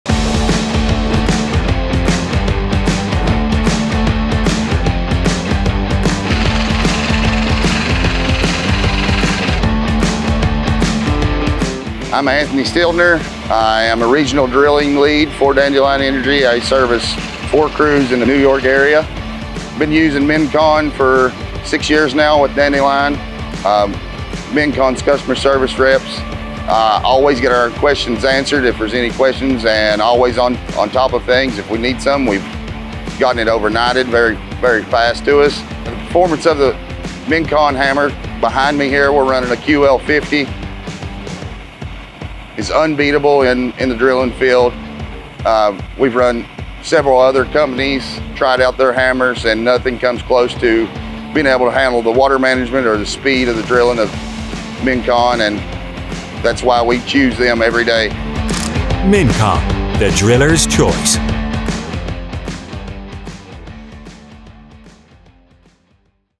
Mincon in Action - Geothermal Drilling with Dandelion Energy
Dandelion Energy in the Northeast USA also trusts Mincon’s drilling tools. Mincon’s hammers and casing systems have been proven to be the right choice for efficient geothermal drilling for Dandelion. This is well summarized on the video by Anthony Stiltner, Dandelion’s regional drilling lead, when he says: “The performance of the Mincon hammer… is unbeatable in the drilling field. And that’s why we choose them every day.”